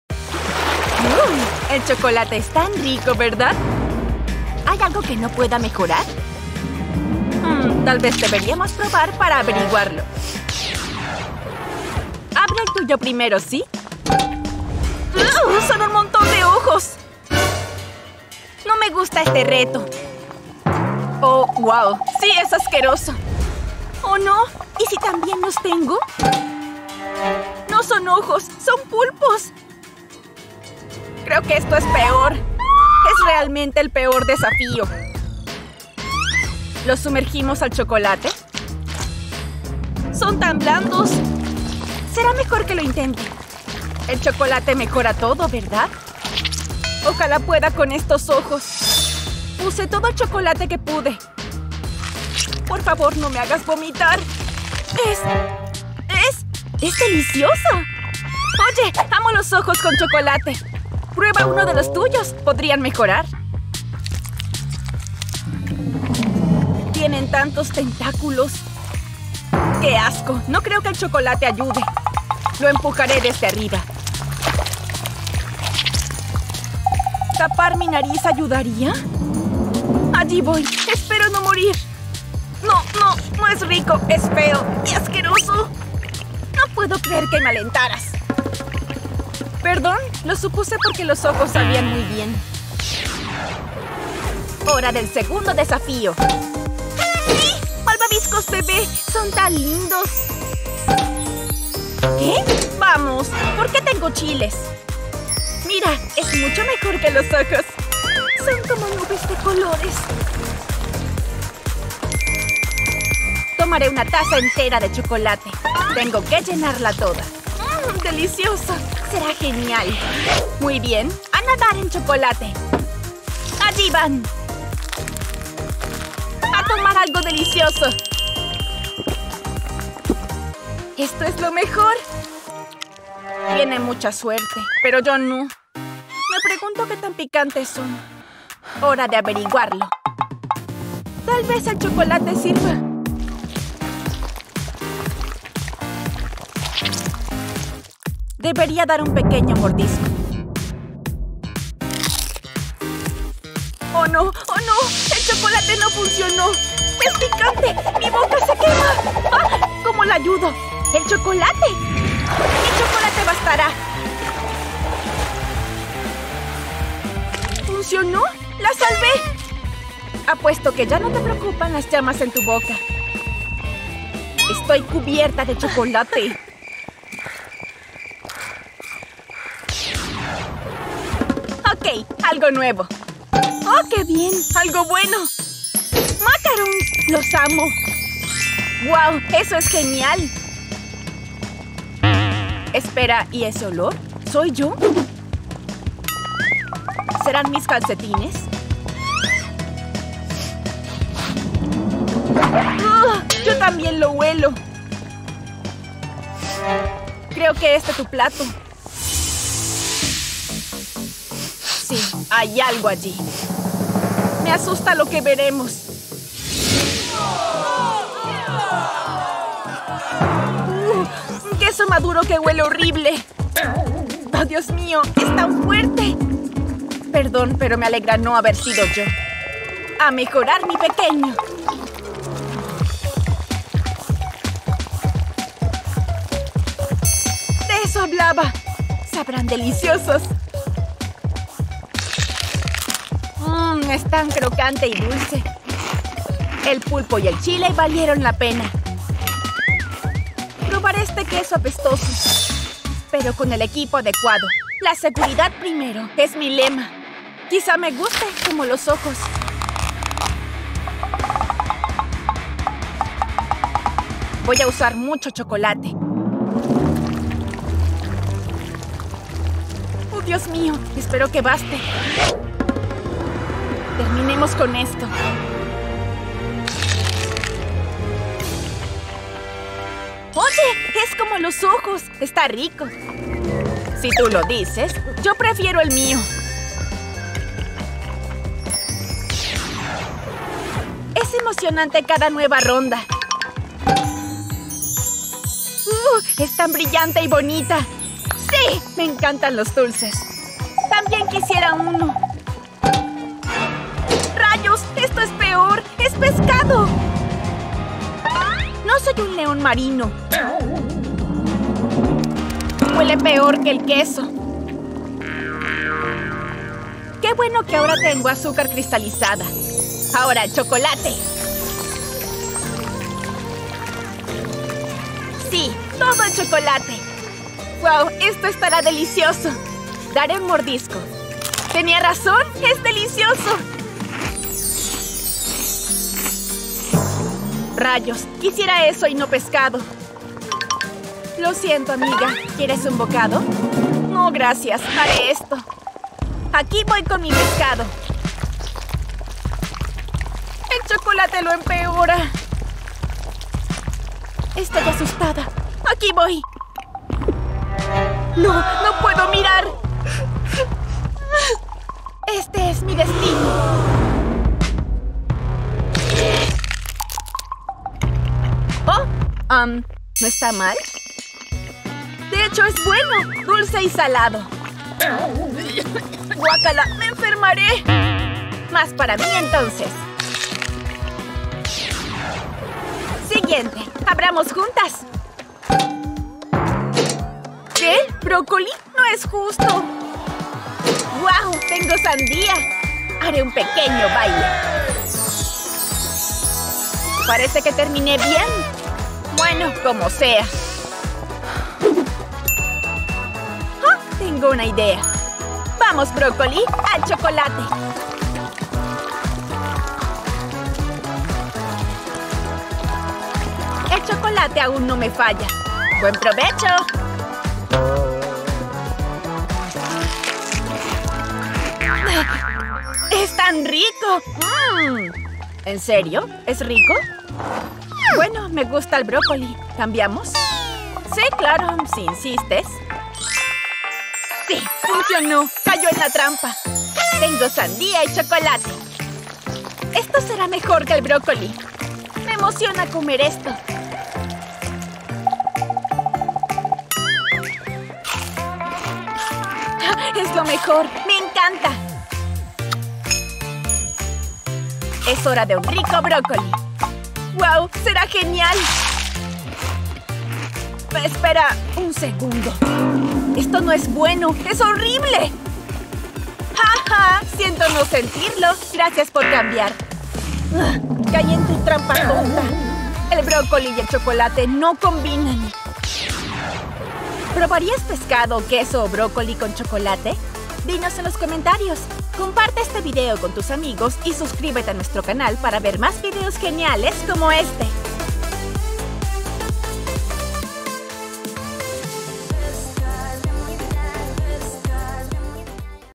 DESAFÍO DE “FONDUE” || ¡LA ÚLTIMA EN PARAR GANA! CHOCOLATE VS. COMIDA REAL POR 123 GO! FOOD
Oooh, el chocolate es tan delicioso, ¿verdad?

¿Probarías pescado, queso o brócoli cubiertos de chocolate? ¡Háznoslo saber en los comentarios!

# 123GO comida # desafío